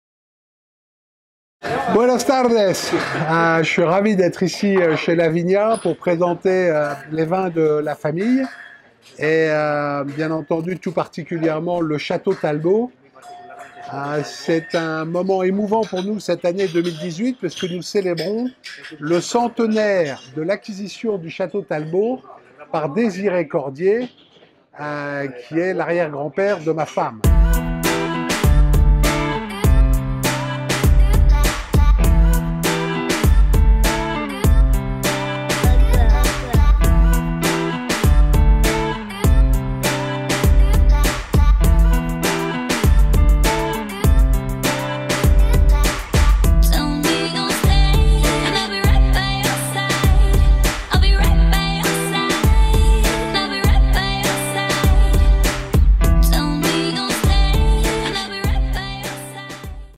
Château Talbot: 100 años de grandes vinos
La Commanderie de Madrid del Grand Conseil de los vinos de Burdeos organizó una cata en Lavinia coincidiendo con el primer centenario de Chàteau Talbot Grand Cru classé de Saint Julian.
Fue dirigida por Jean-Paul Bignon , el propietario, un hombre afable y divertido que hizo aún más especial esta cata.